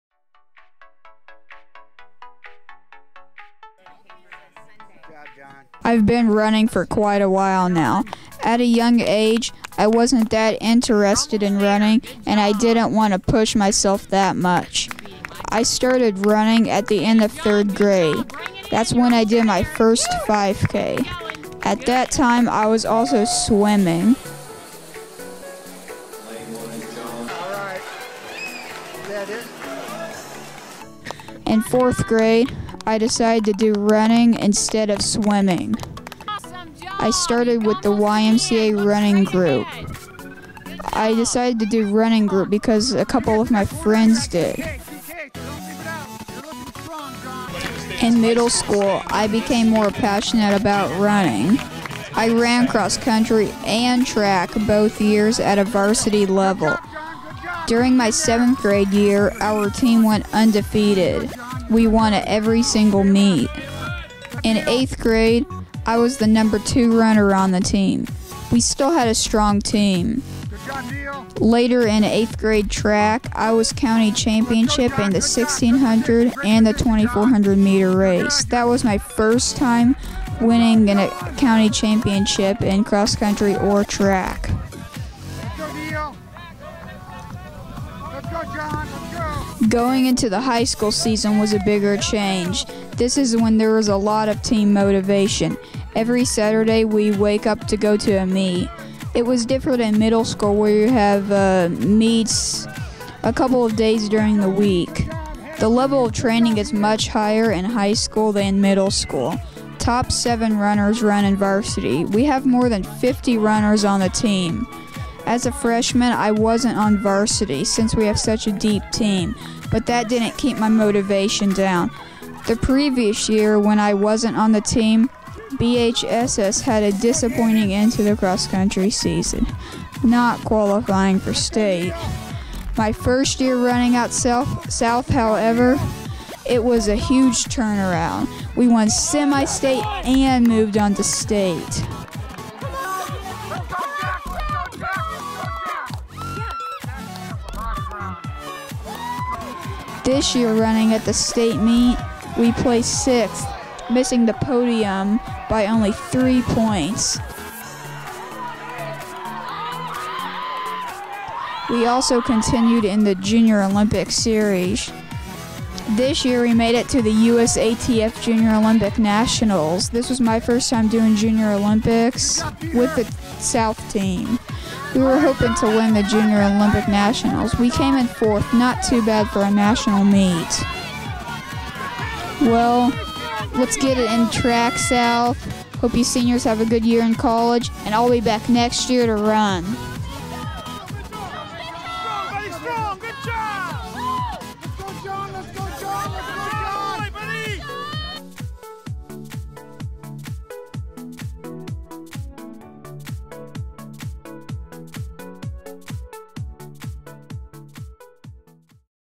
My running career so far!
hey guys, this video is about my passion for running. Watch this to see what my running is like